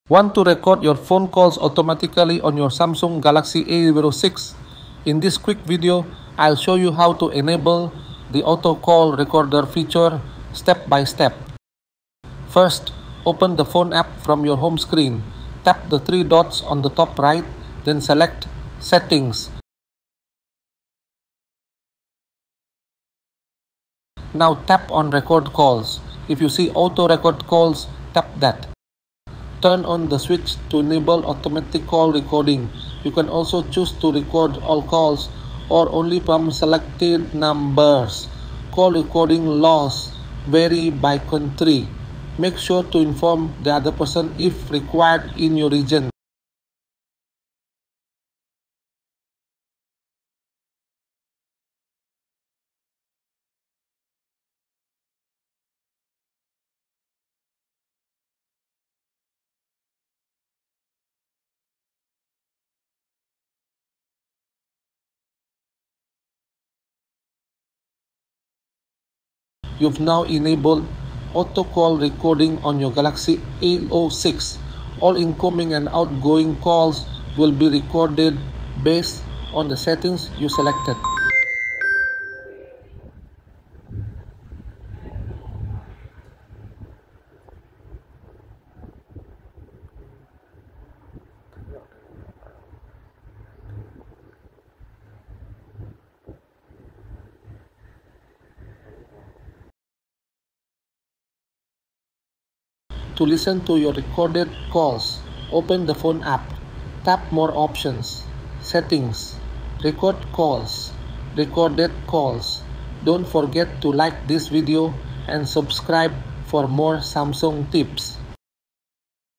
How to Enable Auto Call Recorder on Samsung Galaxy A06
In this video, you’ll learn how to automatically record phone calls on your Samsung Galaxy A06 5G without starting recording manually. This is perfect for saving important conversations, interviews, or personal reminders.

1. Open the Phone app
2. Tap the three-dot menu and select Settings
3. Go to Record calls
4. Tap Auto record calls and switch it ON
(Optional) Choose to record:
- All numbers
- Unsaved numbers only
- Selected numbers

You can even add specific contacts for selective call recording.

⚠️ Note: This feature may not be available in all regions or carriers due to local regulations. Always ensure you follow local laws before recording calls.

- To listen to your recorded calls:
Open the Phone app.
Tap More options ⮕ Settings ⮕ Record calls ⮕ Recorded calls.

- Alternatively:
Open the My Files app.
Navigate to Internal storage ⮕ Call folder to find the recordings in .m4a format.

You have now enabled Auto Call Recording on your Samsung Galaxy A06. All incoming and outgoing calls will be recorded based on the settings you selected.